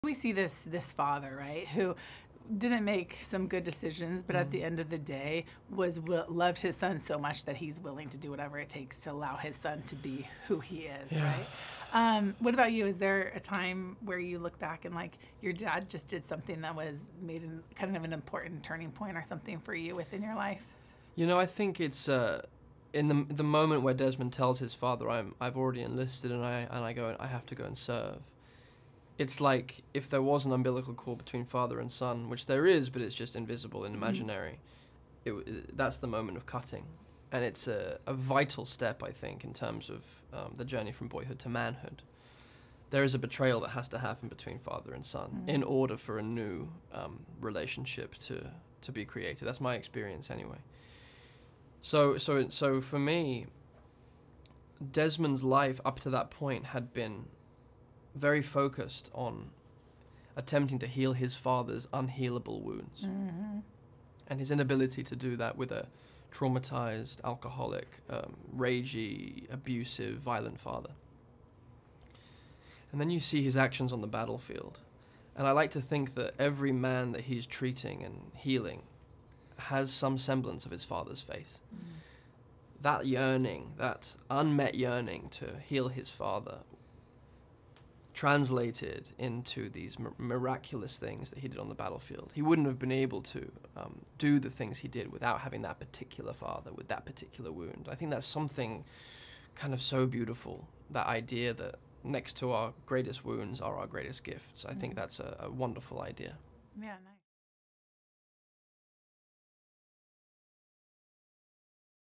Hacksaw Ridge's Andrew Garfield on Father-Son Dynamic
Nominated for six Oscars including Best Picture, Best Director for Mel Gibson and Best Actor for Andrew Garfield, Hacksaw Ridge is the extraordinary true story of WWII army medic Desmond Doss. During one of the bloodiest battles in Okinawa Doss miraculously saved 75 men in a matter of hours without firing or carrying a gun.

Garfield talks about the relationship Doss had with his father and how that ultimately the particular "unmet yearnings" and "wounds" may have been what gave him the capacity to save others.